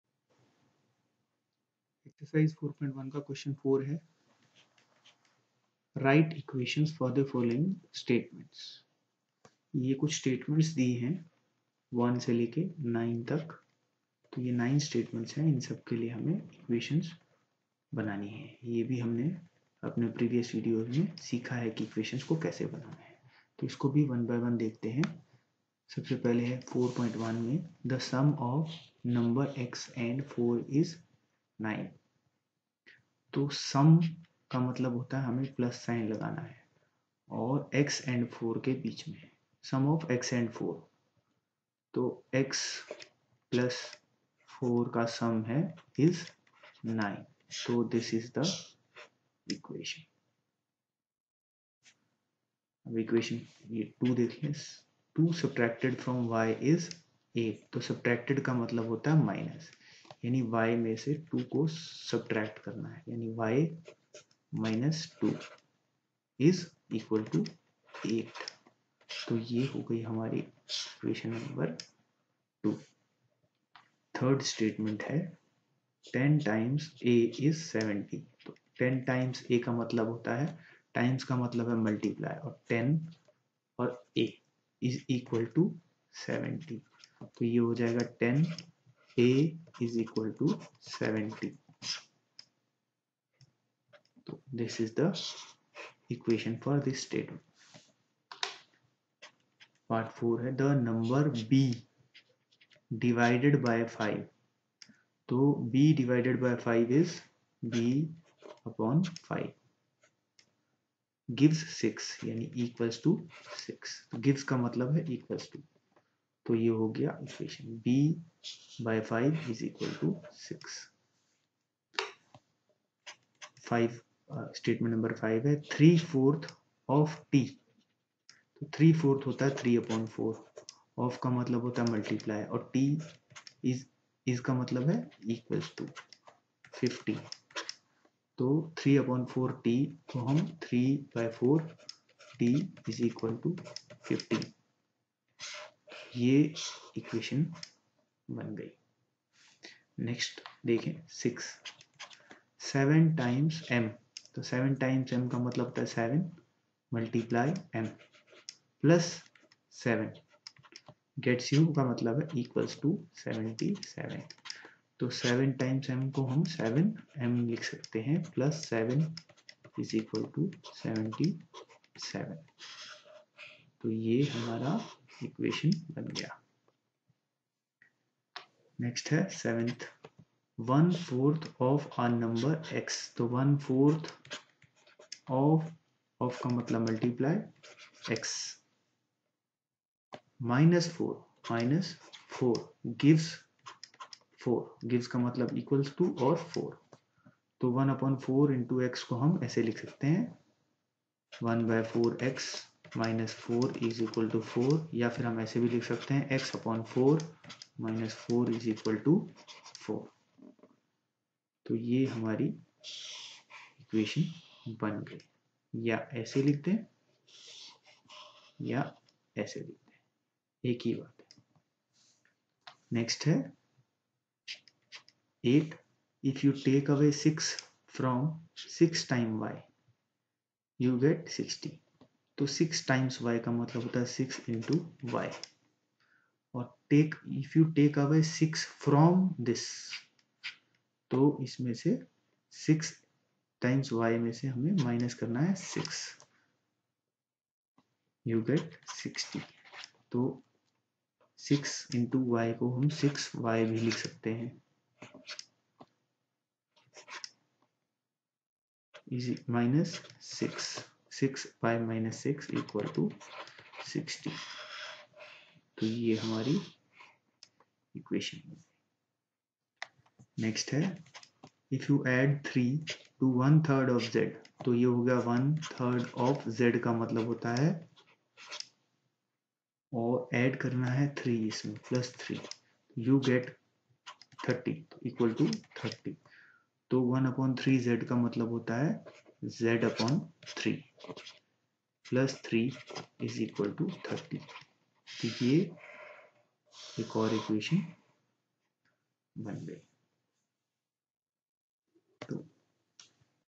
QUESTION 4 EXERCISE 4.1 SIMPLE EQUATIONS Chapter 4 Class 7 Maths, CLASS 7 SIMPLE EQUATIONS
In this video we are going to do the questions of  NCERT and CBSE Class 7 maths Chapter 4, Simple Equations